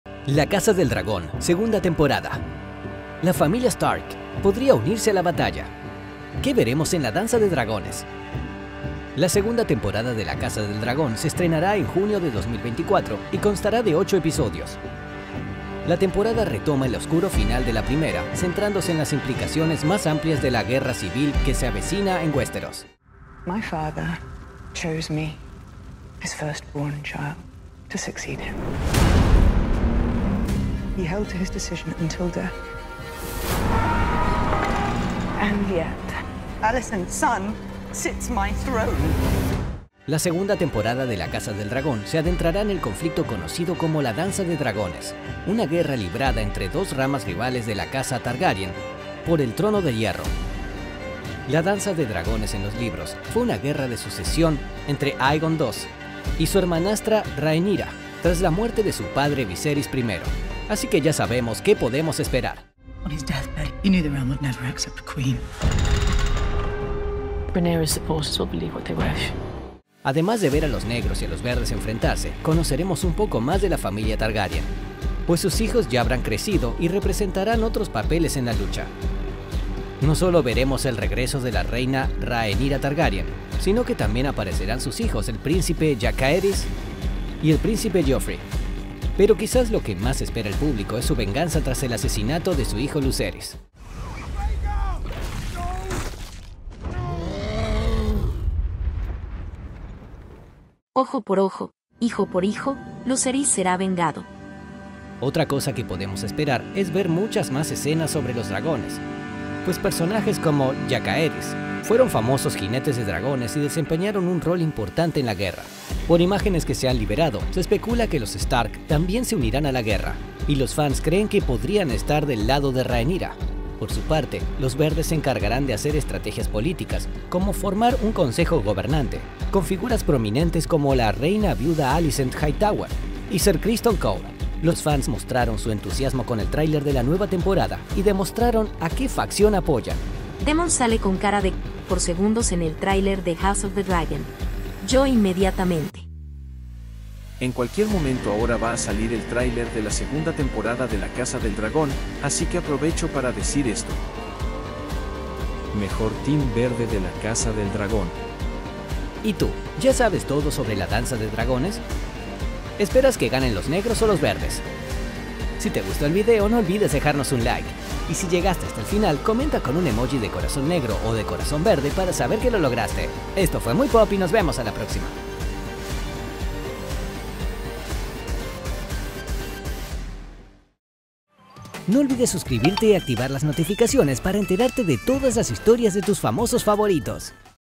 'La casa del dragón': los Stark se unirán a la batalla y otras cosas que veremos en la segunda tempo
'La casa del dragón', segunda temporada: ¿qué veremos en la Danza de dragones? La guerra de los negros contra los verdes, la venganza de Rhaenyra Targaryen y la participación de los Stark.